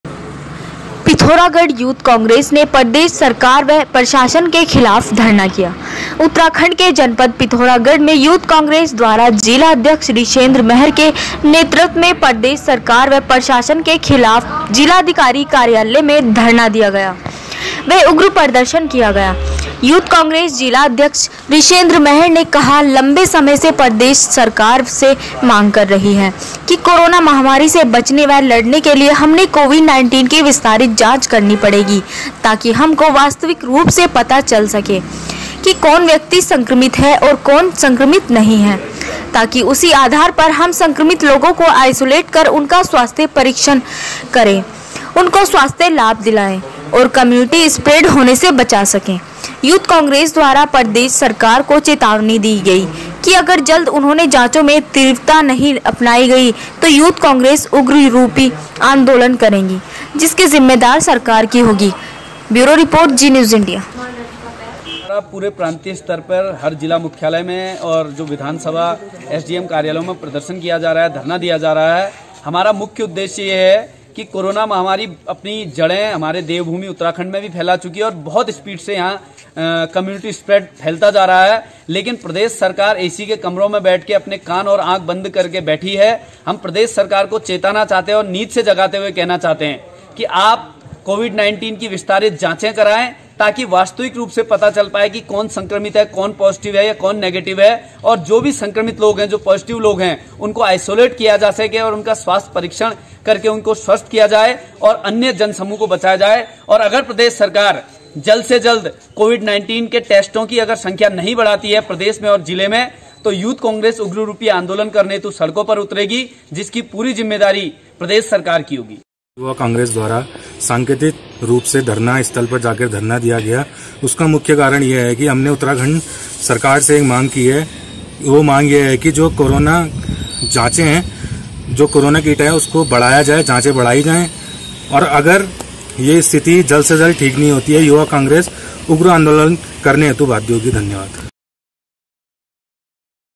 पिथौरागढ यूथ कांग्रेस ने प्रदेश सरकार वह प्रशासन के खिलाफ धरना किया
स्थान उतराखड पिथौरागढ
वीरेंद्र रावत थलीसैंण गढ़वाल

बाइट काला चश्मा यूथ कॉग्रेस जिला अध्यक्ष ऋषेन्द्र महर
बाइट टोपी में यूथ कांग्रेस प्रदेश महासचिव करन सिंह

एंकर उतराखड के जनपद पिथौरागढ में यूथ कांग्रेस द्वारा जिलाध्यक्ष ऋषेन्द्र महर के नेतृत्व मे प्रदेश सरकार व प्रशासन के खिलाफ जिलाधिकारी कार्यालय में धरना दिया गया व उग्र प्रदर्शन किया गया। यूथ कांग्रेस जिला अध्यक्ष  ऋषेन्द्र महर ने कहा लंबे समय से प्रदेश सरकार से मांग कर रही है कि कोरोना महामारी से बचने व लड़ने के लिए हमने कोविड-19 की विस्तारित जांचे करनी पड़ेगी ताकि हमको वास्तविक रूप से पता चल सके कि कौन व्यक्ति संक्रमित है और कौन संक्रमित नहीं है, ताकि उसी आधार पर हम संक्रमित लोगों को आइसोलेट कर उनका स्वास्थ्य परीक्षण करें उनको स्वास्थ्य लाभ दिलाएं और कम्युनिटी स्प्रेड होने से बचा सके।
यूथ कांग्रेस द्वारा प्रदेश सरकार को चेतावनी दी गई कि अगर जल्द उन्होंने जाँचो में तीव्रता नहीं अपनाई गई तो यूथ कांग्रेस उग्र रूपी आंदोलन करेगी जिसकी जिम्मेदार सरकार की होगी!